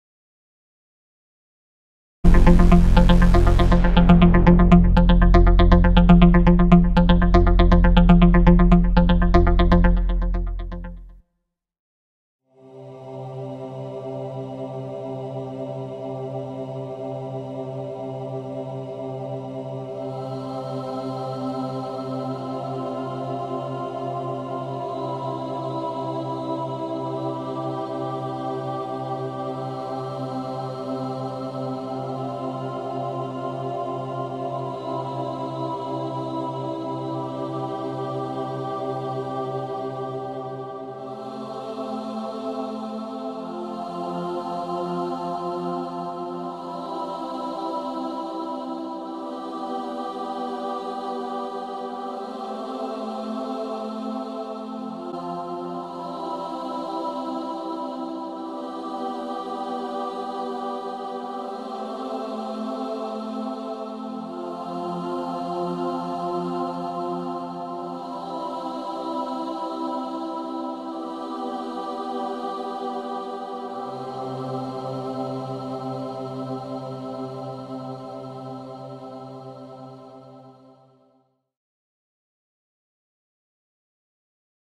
ROLEX Close Up Macro Milgauss 116400GV Z Blue Oyster 40mm
Detailed macro of the Rolex Milgauss Z-Blue